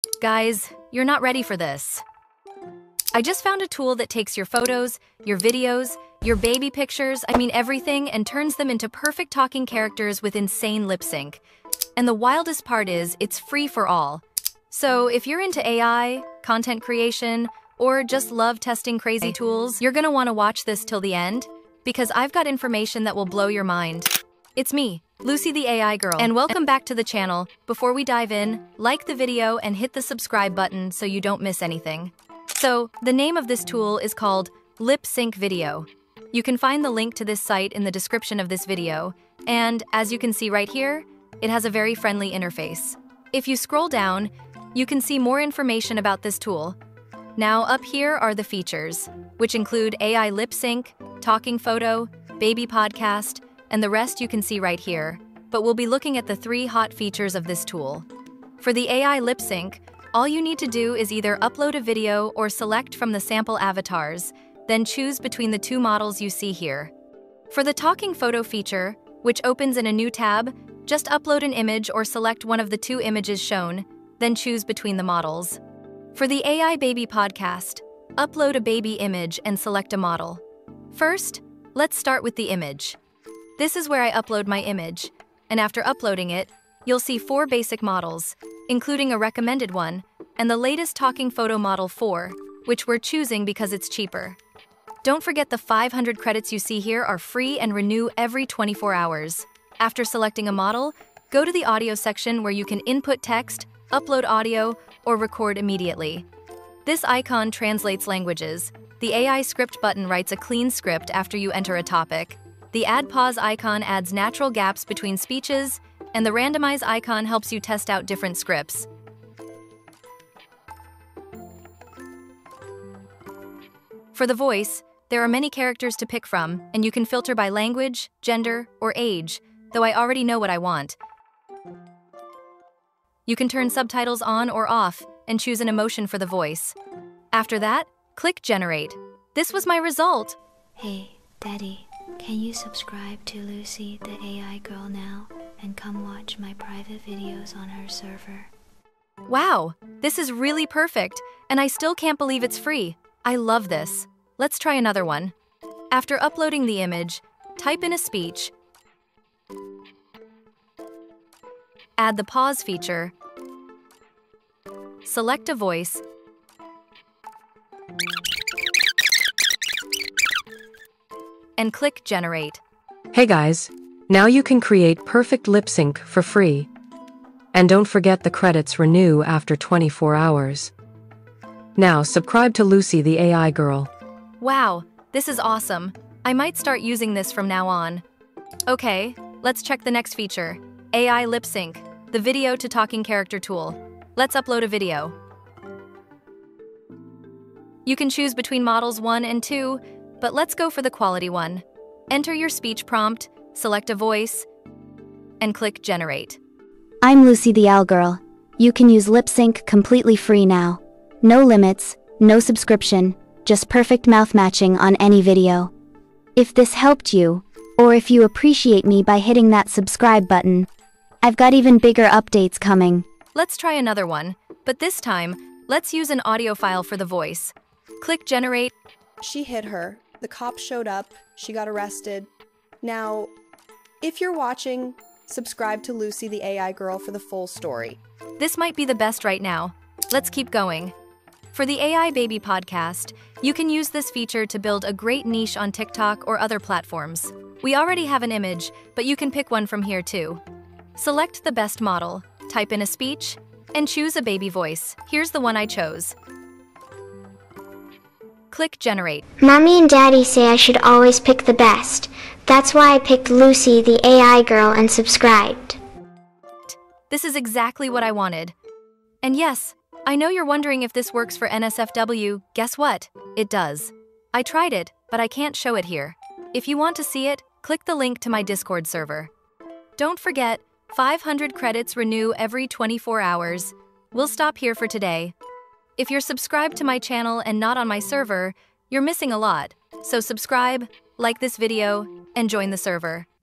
I'm an Expert and I Just Found Out About This FREE AI Lip Sync Tool
You can now turn ANY photo, video, or even a baby picture into a perfect talking character with ultra-realistic lip sync — completely FREE using LipSync.video. In this tutorial, I’ll show you how to generate talking photos, talking videos, AI baby podcasts, and full lip-synced characters in minutes using one powerful tool that requires no subscription, no region restrictions, and gives you 500 free credits every 24 hours.

LipSync.video is famous for its accuracy, its natural mouth movements, emotion-based voices, and its powerful text-to-speech model… But the best part?
It is 100% FREE and renews daily.

Today, you can finally take advantage of everything inside the platform.

With this tool, you can:

✅ Turn any photo into a talking character
✅ Lip-sync full videos with custom scripts
✅ Upload your own audio and match it perfectly
✅ Create AI baby podcast clips for TikTok
✅ Generate clean scripts automatically using AI
✅ Add pauses and emotions to make speech natural
✅ Use multiple languages + auto-translation
✅ Export in HD
✅ Use LipSync.video FREE — no subscription required

🎁 BONUS: I’ll also show you how to use the hidden tools inside LipSync.video, including random script generation, language auto-translation, and the newest Talking Photo 4.0 model.

In this tutorial, you’ll learn:

– How to use LipSync.video step-by-step
– How to create talking images in seconds
– How to lip-sync entire videos
– How to upload your own voice or audio
– How to create viral baby podcast clips
– How to pick the perfect AI model
– How to maximize your 500 daily credits
– How to use the “Add Pause” tool the right way
– How to build content for TikTok, YouTube & IG